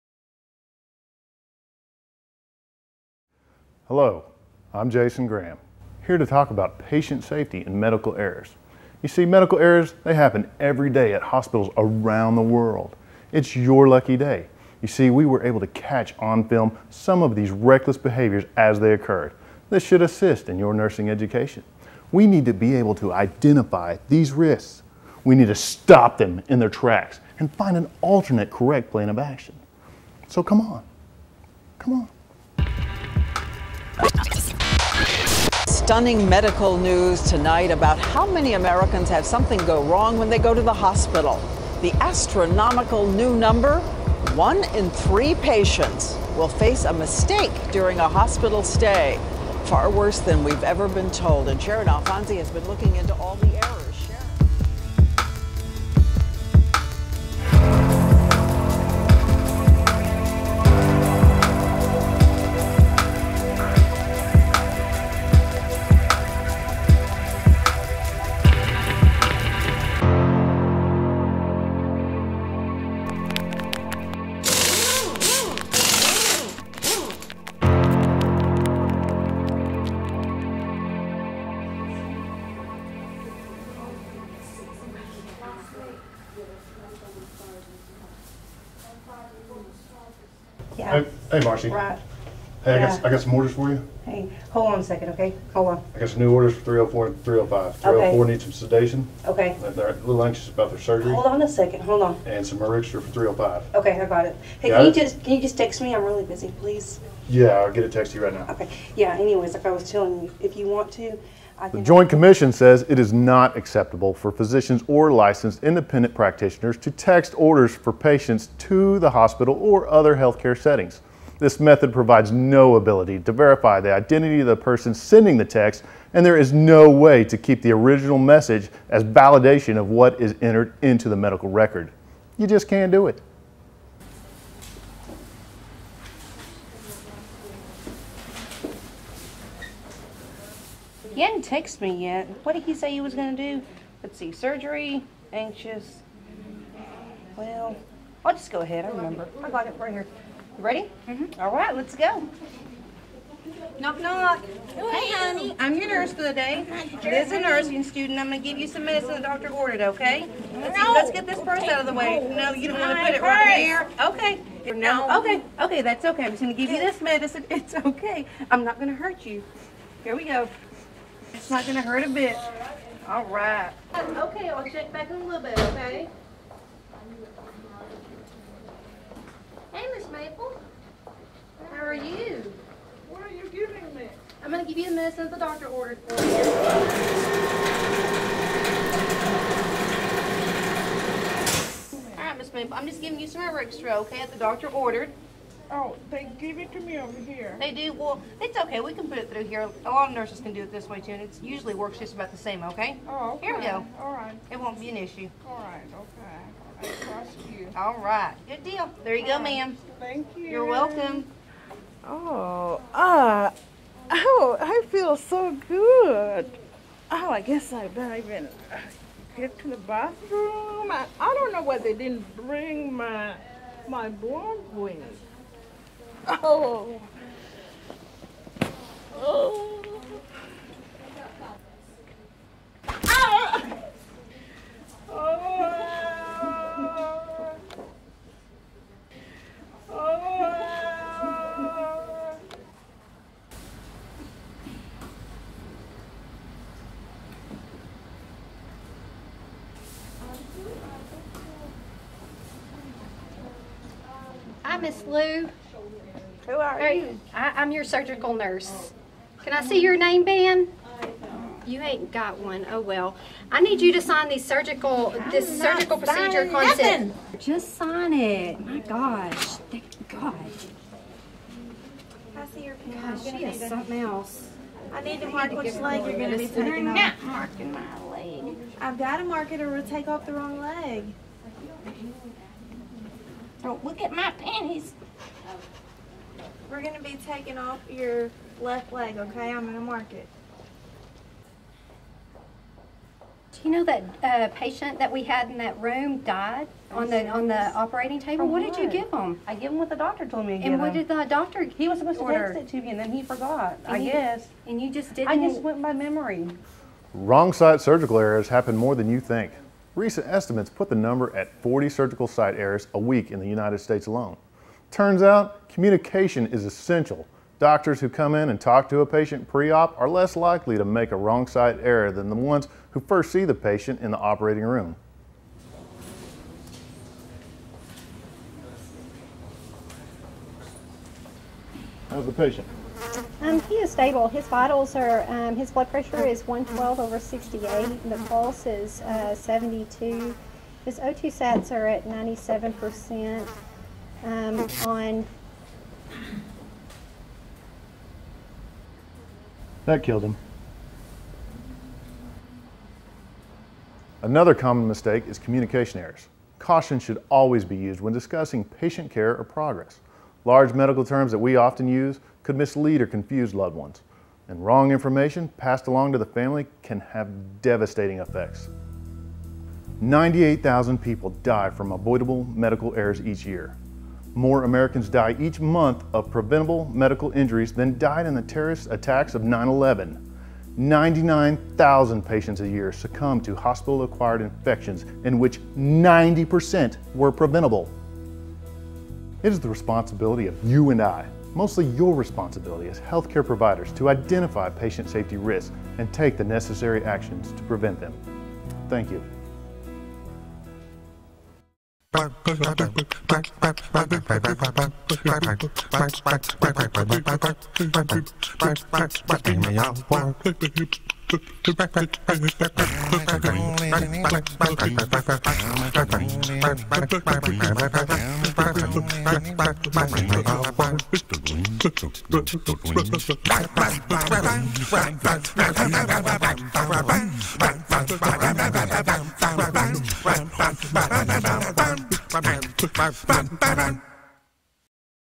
Patient Safety Healthcare Errors
A Nursing school group project video to show the errors that happen in a health care setting.  2012